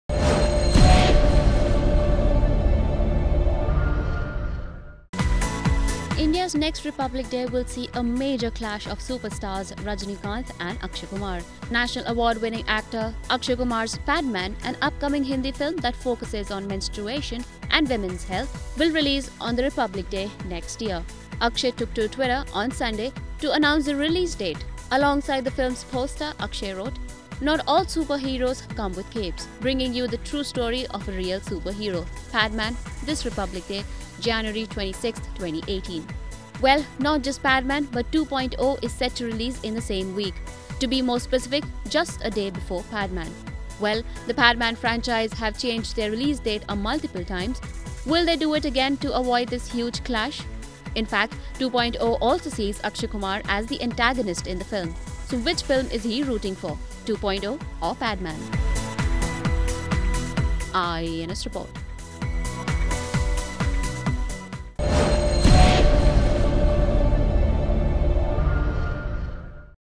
Akshay Kumar CLASHES with Rajinikanth | 2.0 v/s Padman
India's next Republic day will see a major clash of superstars Rajinikanth and Akshay Kumar. National Award winning actor Akshay Kumar's "Padman", an upcoming Hindi film that focuses on menstruation and women's health, will release on the Republic Day next year.